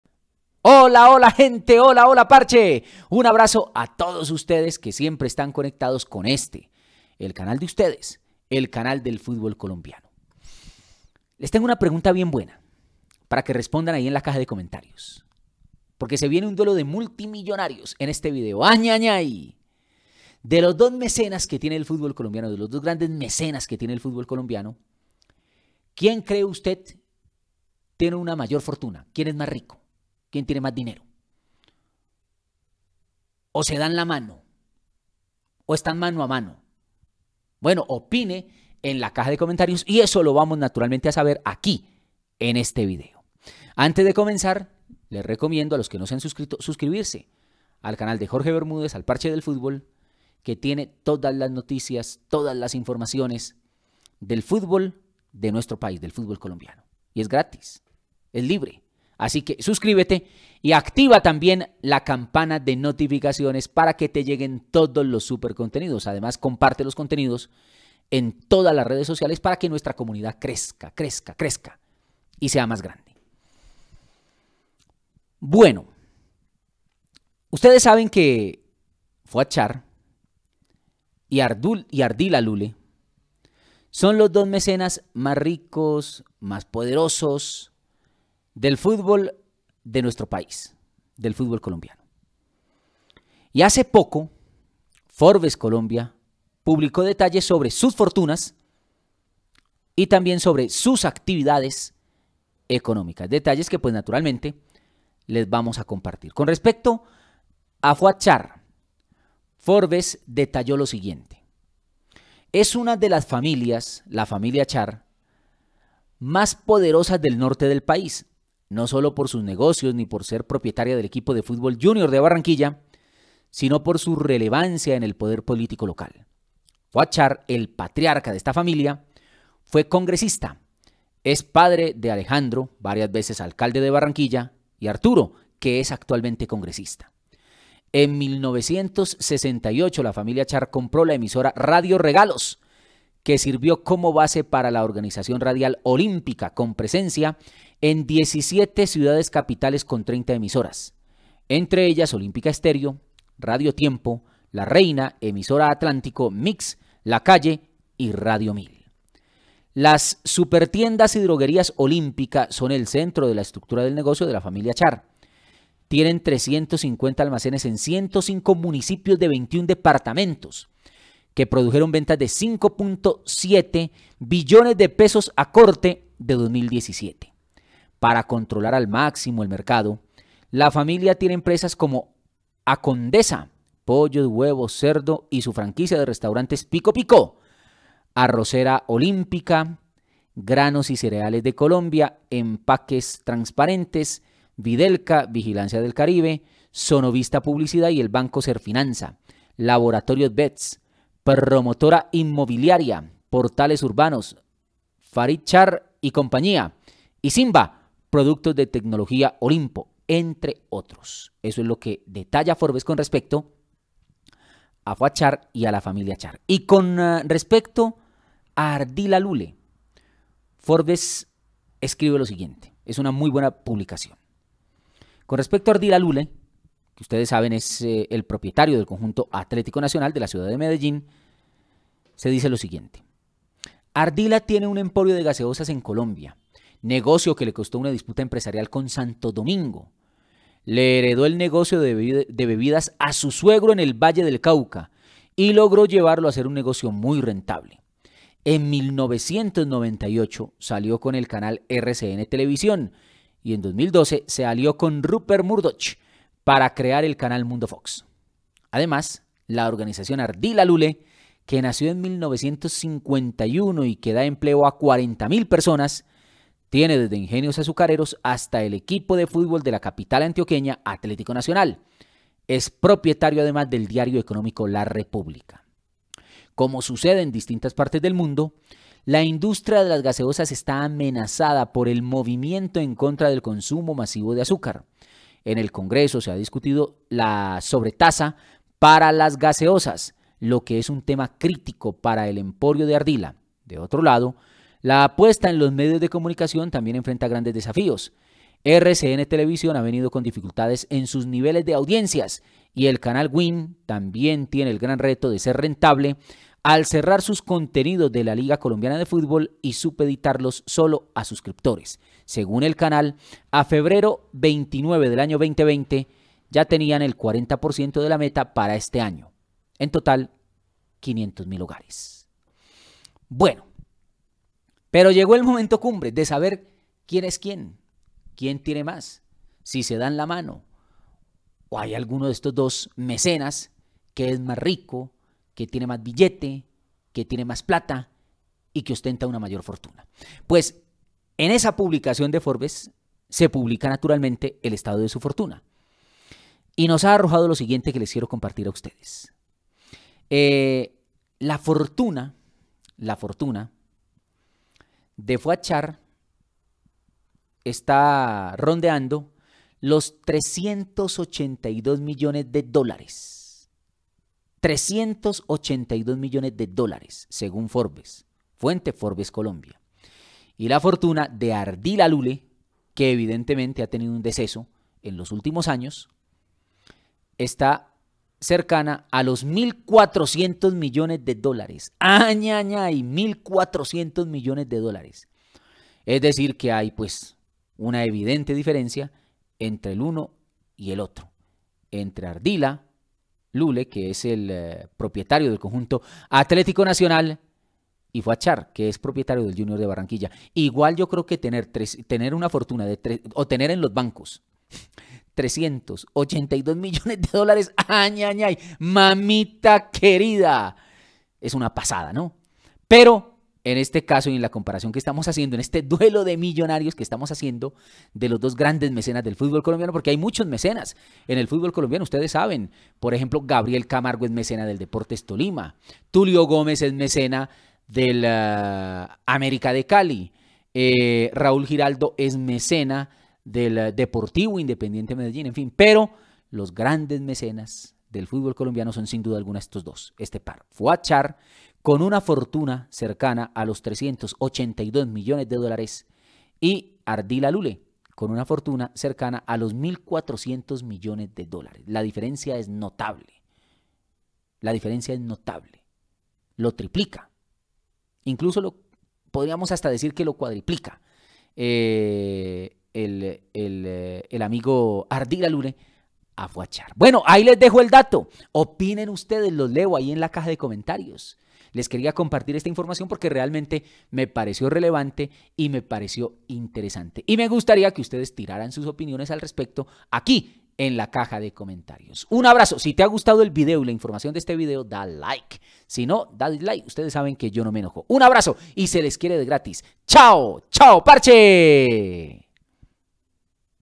FUAD CHAR Ó ARDILA LULLE: QUIÉN TIENE LA MAYOR FORTUNA ENTRE LOS MECENAS DEL FÚTBOL DE COLOMBIA?
Información con Jorge Bermúdez...